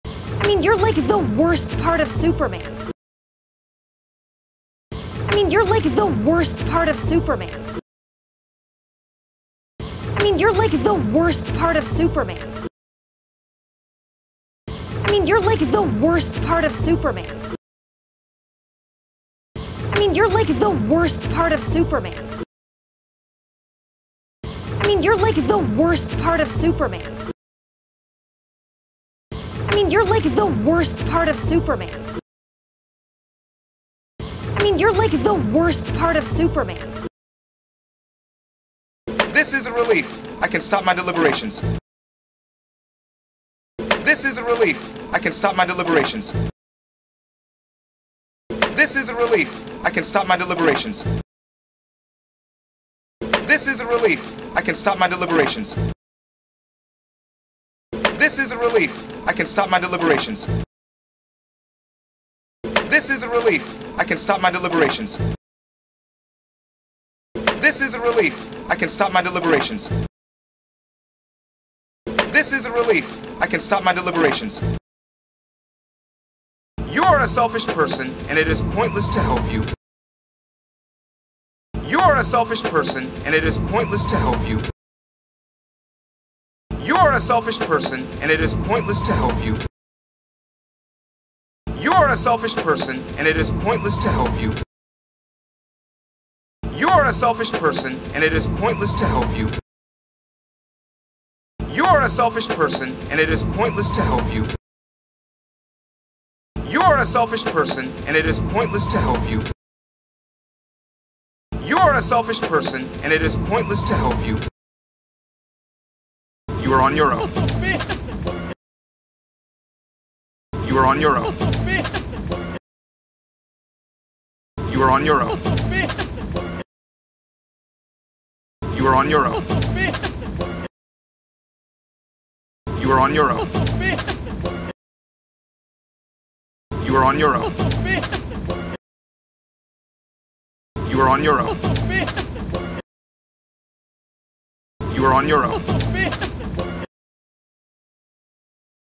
[제128강3] SHADOWING 훈련
SHADOWING 훈련용입니다. 시즌1x2회 영상입니다.

문장별로 8회 반복되며 말을 따라 해보는 SHADOWING 훈련 콘텐츠입니다.

자막이 없고요. LISTENING 훈련을 충분히 하신 후에 훈련하는 것이 좋습니다.